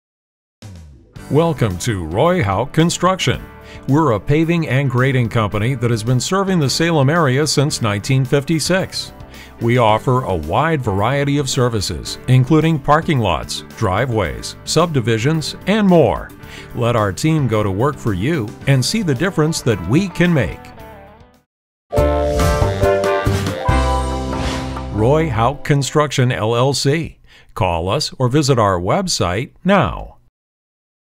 Roy Houck Construction, LLC.
Welcome to Roy Houck Construction! Were a paving and grading
company that has been serving the Salem area since 1956. We
offer a wide variety of services, including parking lots,
driveways, subdivisions, and more. Let our team go to work
for you, and see the difference that we can make!